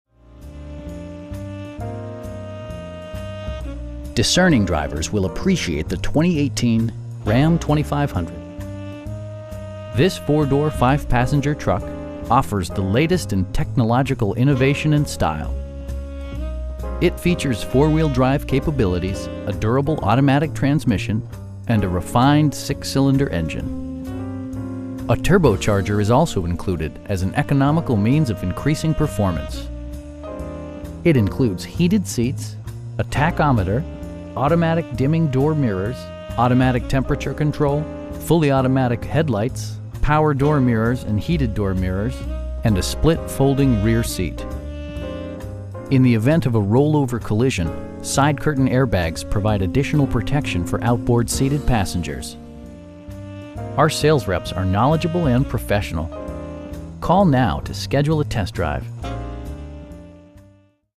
2018 Ram 2500 LONE STAR CREW CAB 4X4 6'4 BOX
All American Chrysler Dodge Jeep Ram of Odessa
2510 East 8th Street

Discerning drivers will appreciate the 2018 Ram 2500. This 4 door, 5 passenger truck offers the latest in technological innovation and style. It features four-wheel drive capabilities, a durable automatic transmission, and a refined 6 cylinder engine. A turbocharger is also included as an economical means of increasing performance. It includes heated seats, a tachometer, automatic dimming door mirrors, automatic temperature control, fully automatic headlights, tilt steering wheel, power door mirrors and heated door mirrors, and a split folding rear seat. In the event of a rollover collision, side curtain airbags provide additional protection for outboard seated passengers. Our sales reps are knowledgeable and professional. They'll work with you to find the right vehicle at a price you can afford.